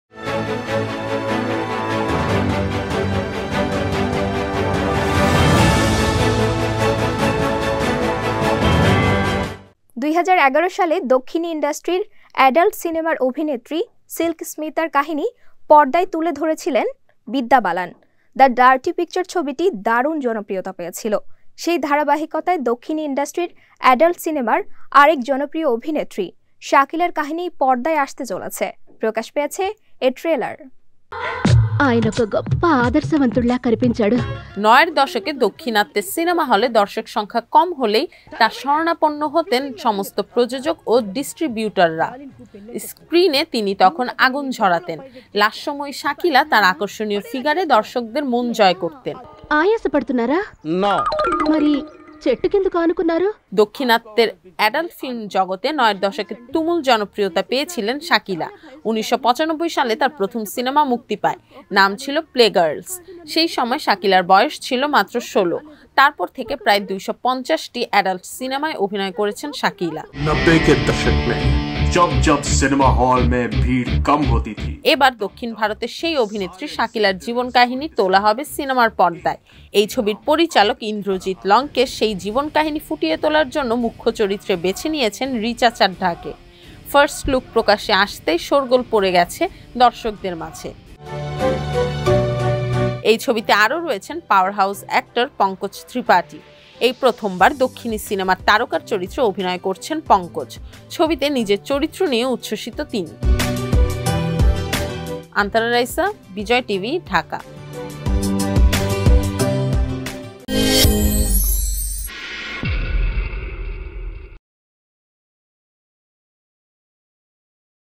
অ্যাডাল্ট ছবির নায়িকা শাকিলাকে নিয়ে ছবি ‘শাকিলা’ । Bijoy TV
২০১১ সালে দক্ষিণী ইন্ডাস্ট্রির অ্যাডাল্ট সিনেমার অভিনেত্রী সিল্ক স্মিতার কাহিনি পর্দায় তুলে ধরেছিলেন বিদ্যা বালান । ‘দ্য ডার্টি পিকচার’ ছবিটি দারুণ জনপ্রিয়তা পেয়েছিল । সেই ধারাবাহিকতায় দক্ষিণ ইন্ডাস্ট্রির এডাল্ট সিনেমার আরেক জনপ্রিয় অভিনেত্রি শাকিলার কাহিনী পর্দায় আসতে চলেছে। প্রকাশ পেয়েছে এর ট্রেইলার।